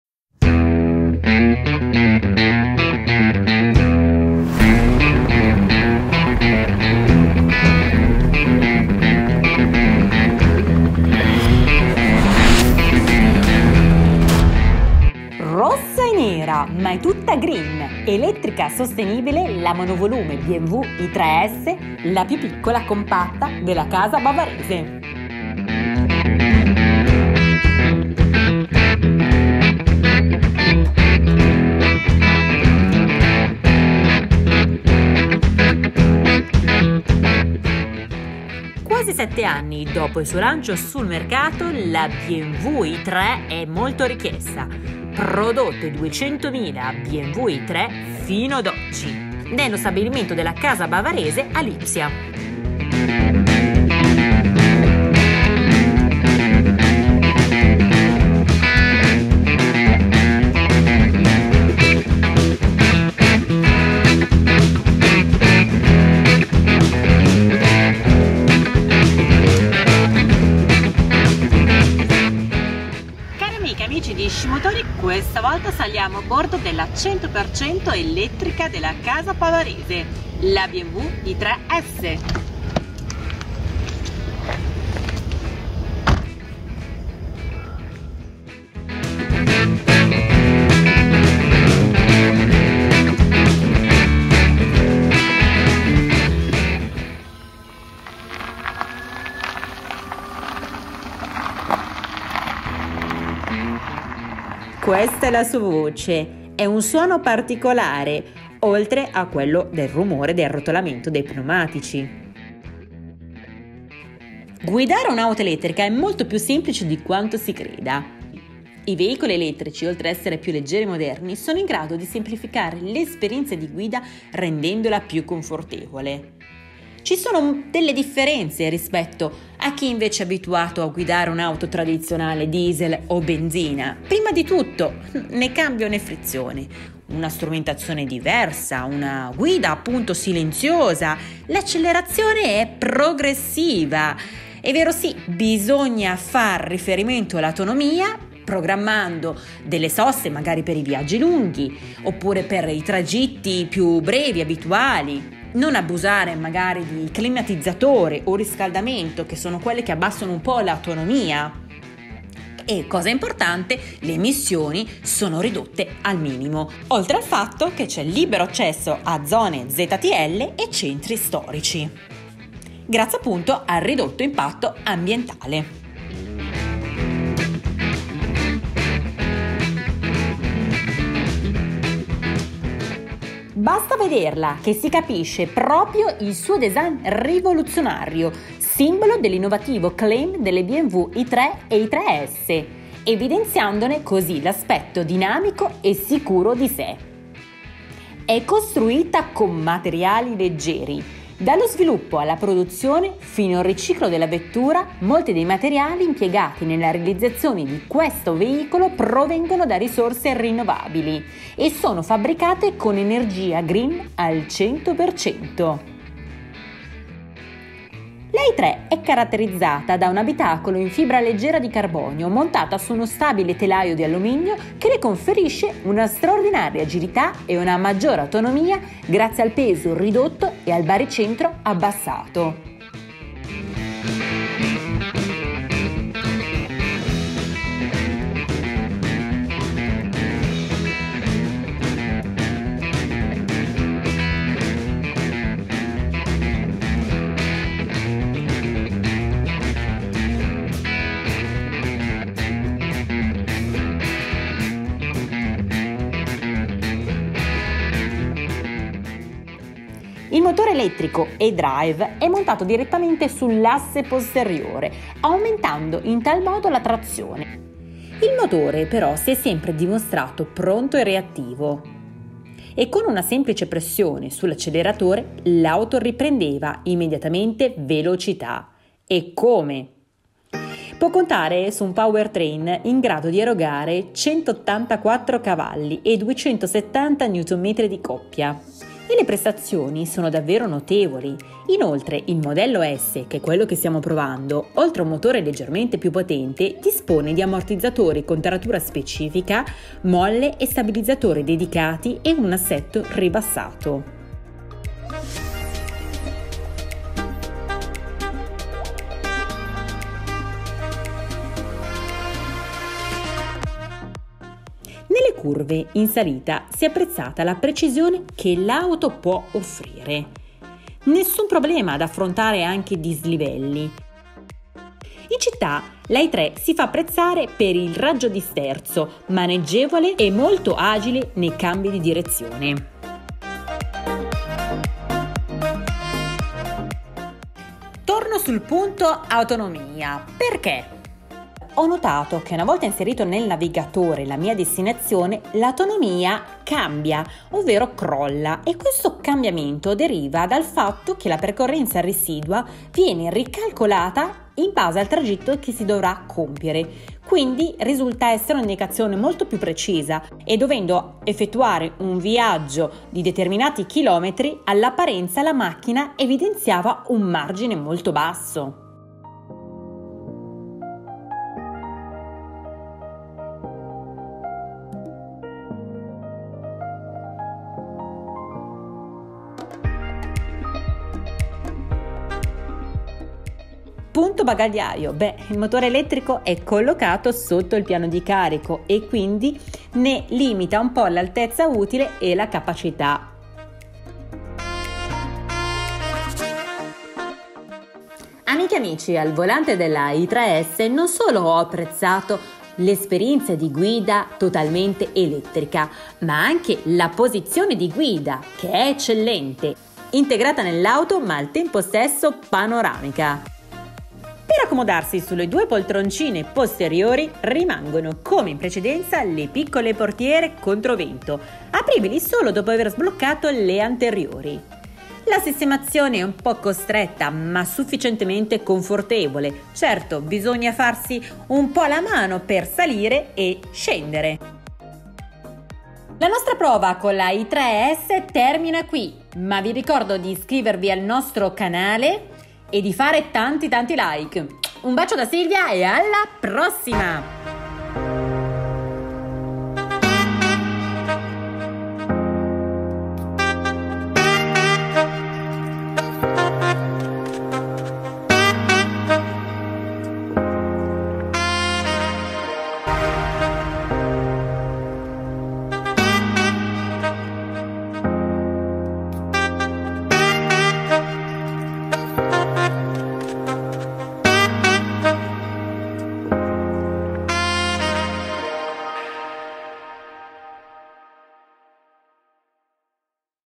BMW I3S
Siamo Salite a bordo della più piccola della Casa Bavarese la BMWi3S elettrica e sostenibile.
Dallo sviluppo alla produzione fino al riciclo della vettura, molti dei materiali impiegati nella realizzazione  provengono da risorse rinnovabili.
Caratterizzata da un abitacolo in fibra leggera di carbonio è agile viaggia nel silenzio quasi assoluto e senza emissioni.
Ecco la nostra prova ;)